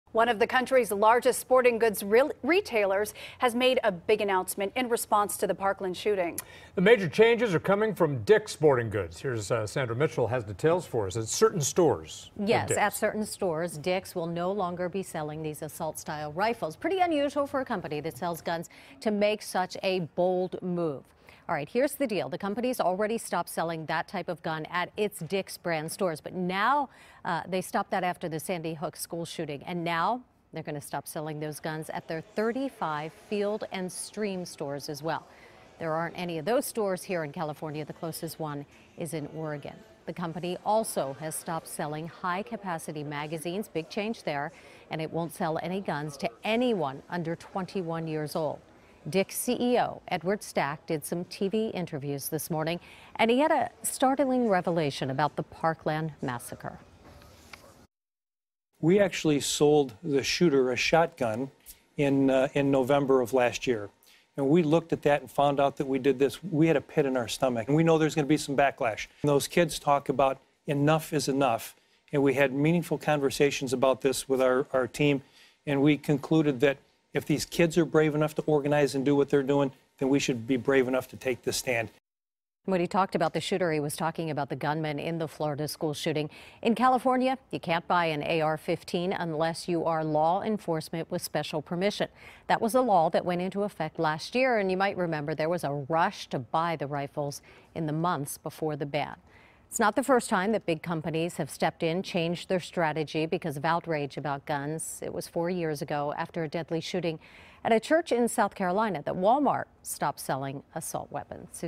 Dick's Drops Assault-Style Rifle Sales
The company will also stop selling high-capacity magazines and selling guns to anyone under 21. Sandra Mitchell reports.